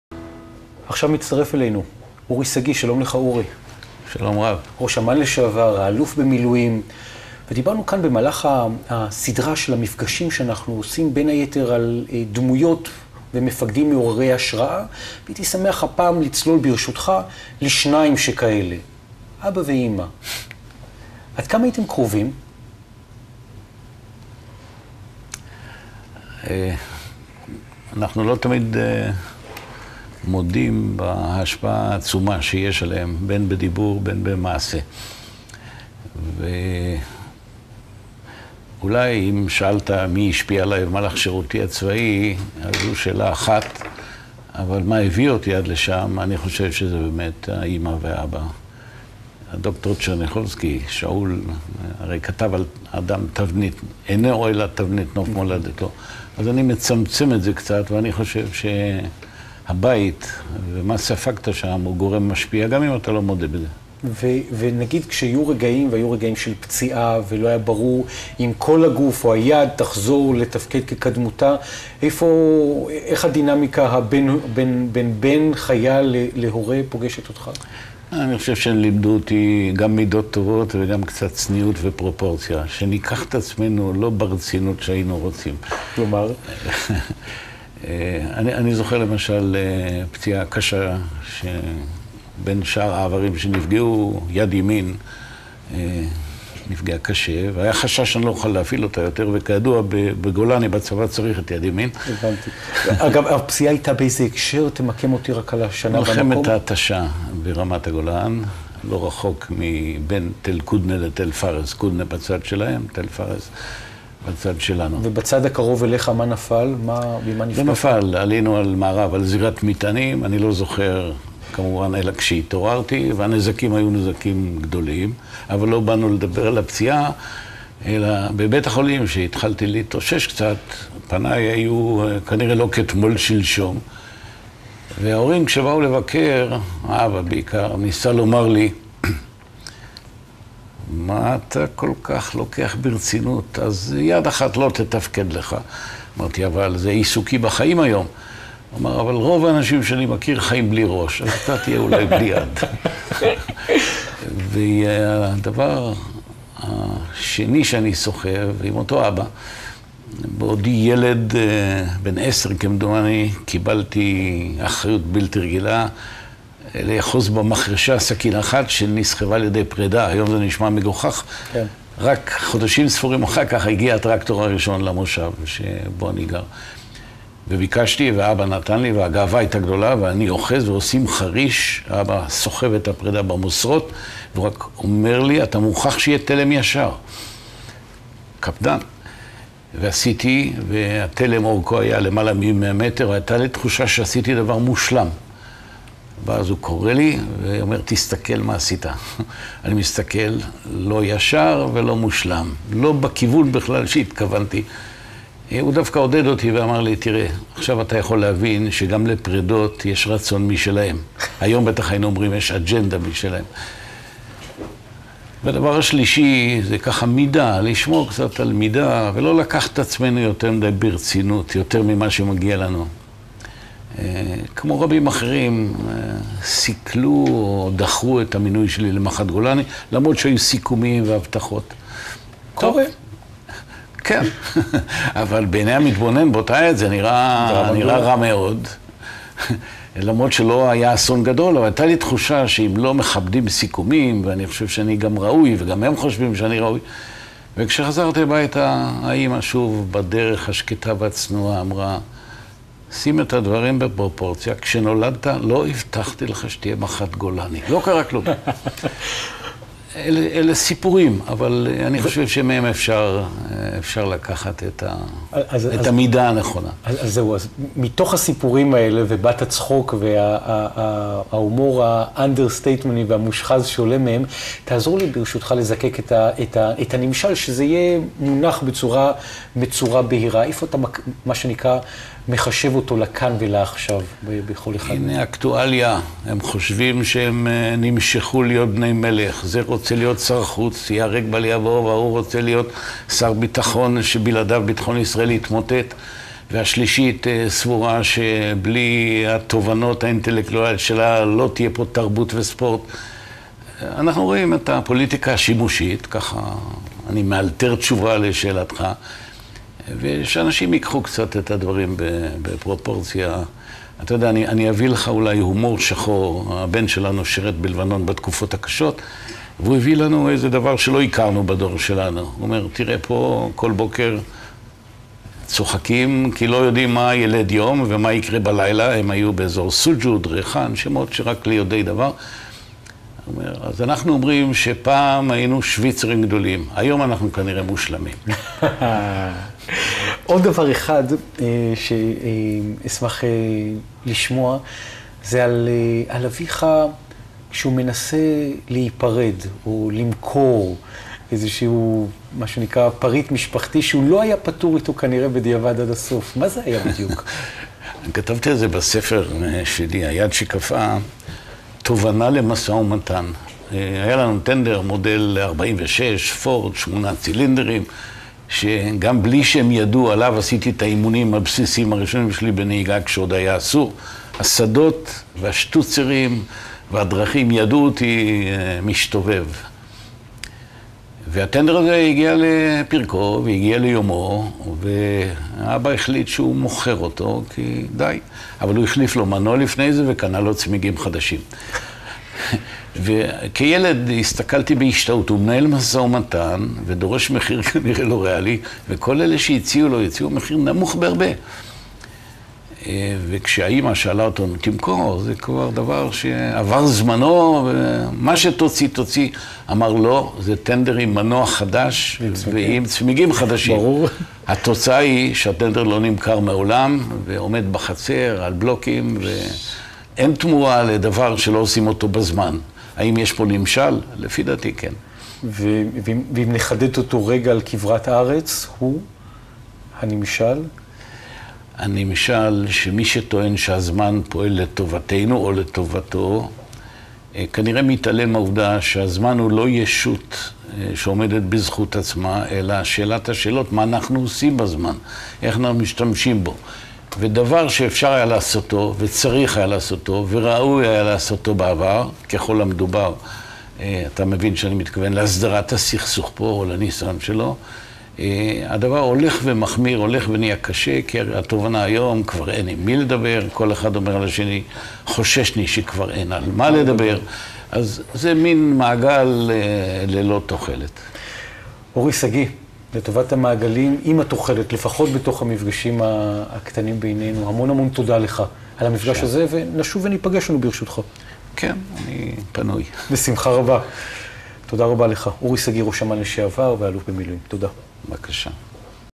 אורי שגיא בראיון עם נתנאל סמריק באולפן הטלוויזיה של קונטנטו
אורי שגיא בראיון עם נתנאל סמריק באולפן הטלוויזיה של קונטנטו, בית ההוצאה לאור הבינלאומי. השידור הוא בערוץ הטלוויזיה ContentoTv , שידור גם בערוץ 10. רוצים לראות עוד סיפורים שיוצאים לאור ? מוזמנים